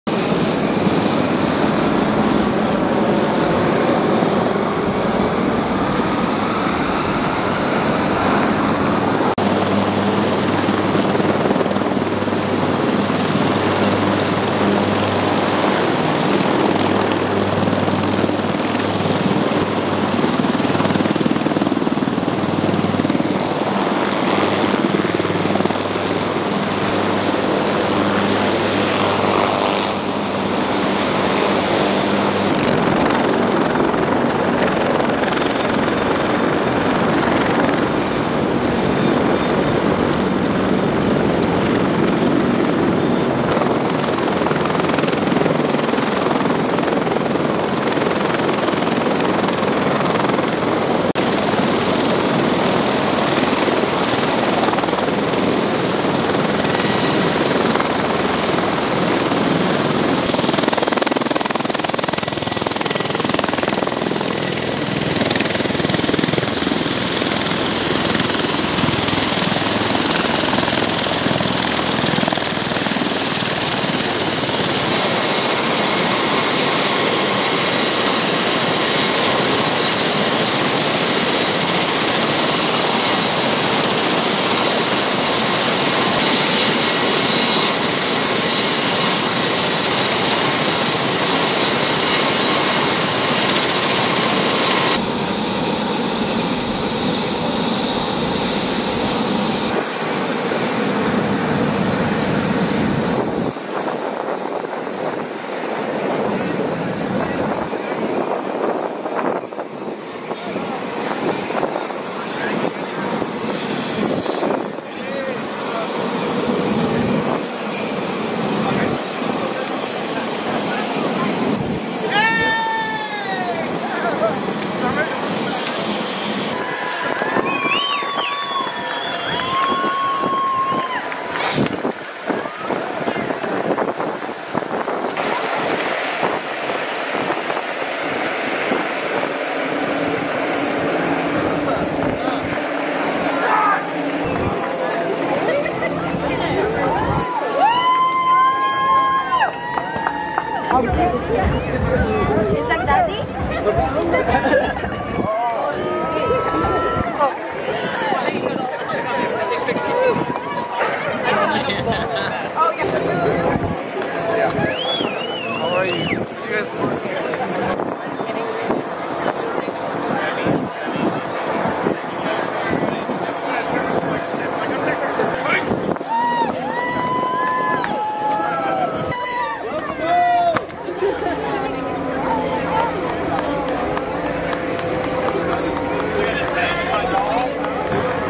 05.22.2021: U.S. Marines and Sailors with the 3rd Marine Air Wing, VMM-164 Homecoming
VMM-164 Homecoming
CA, UNITED STATES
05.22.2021

U.S. Marines and Sailors with the 3rd Marine Air Wing, Medium Tiltrotor Squadron 164 (VMM-164) reunite with their families on May 22, 2021 on Camp Pendleton, California. The Marines and Sailors of the 15th Marine Expeditionary Unit, embarked aboard Makin Island Amphibious Ready Group, returned home after a seven month deployment serving as a crisis-response force for combatant commanders in the Africa, Central, and Indo-Pacific commands. (U.S. Marine Corps video by Lance Cpl. Garrett Kiger)

Film Credits: Video by Lance Cpl. Garrett Kiger
I Marine Expeditionary Force

Camp Pendleton
15th MEU
3rd MAW
Sailors
Marines
VMM-164